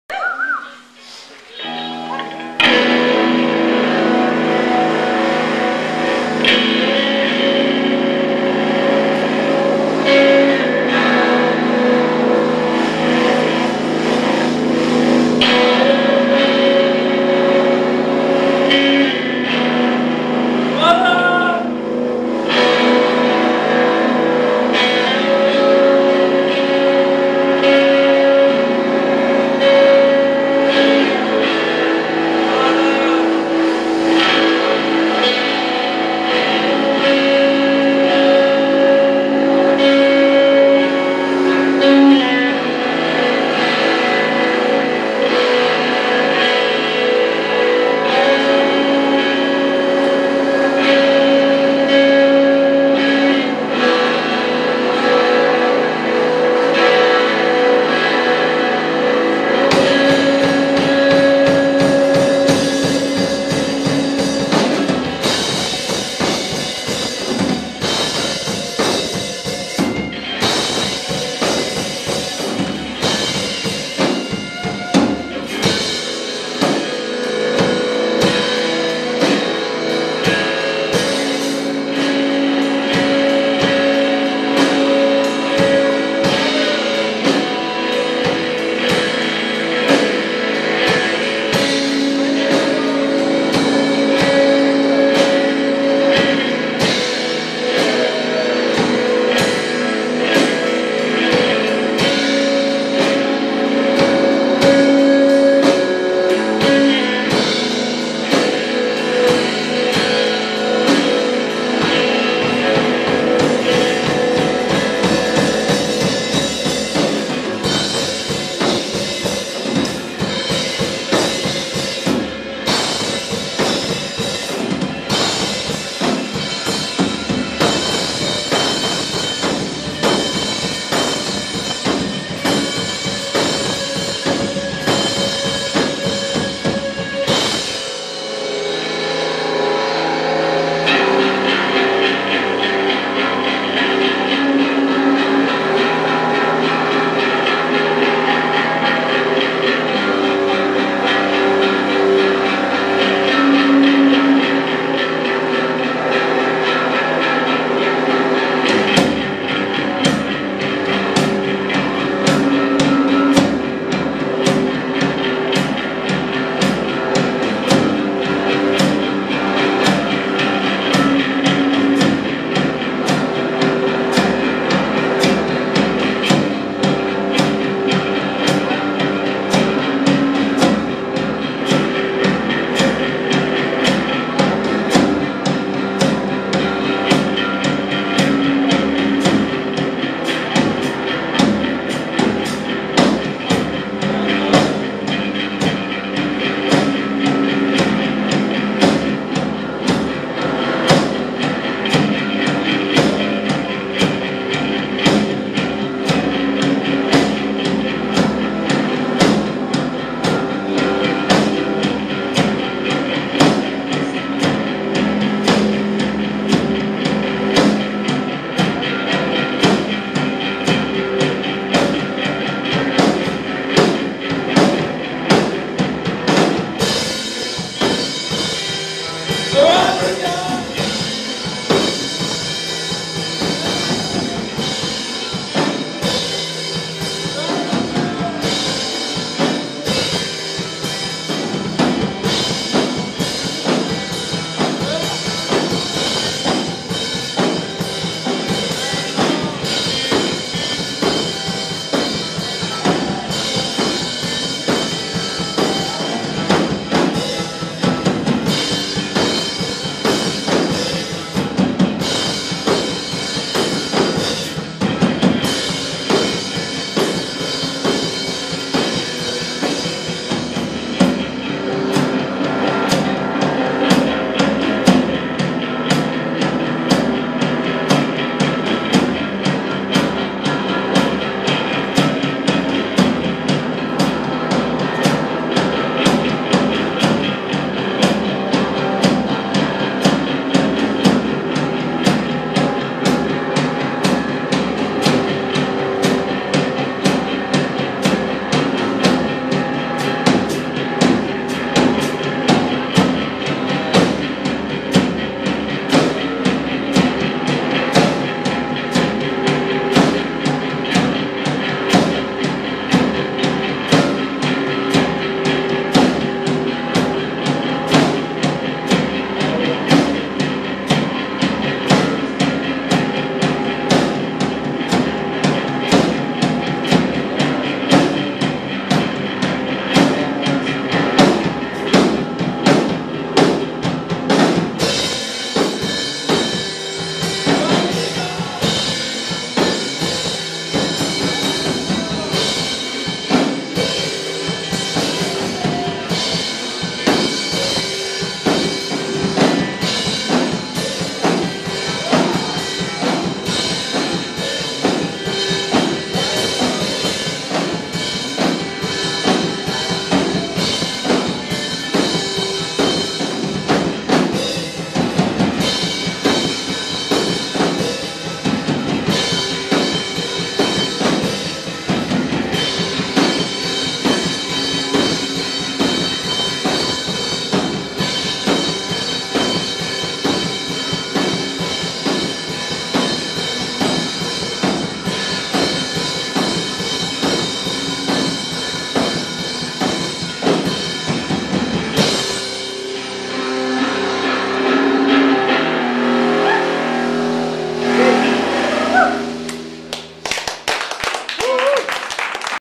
Random Veracity
A random early 2013 Veracity show. If anybody knows who the band is, leave me a message. I like to give appropriate credit.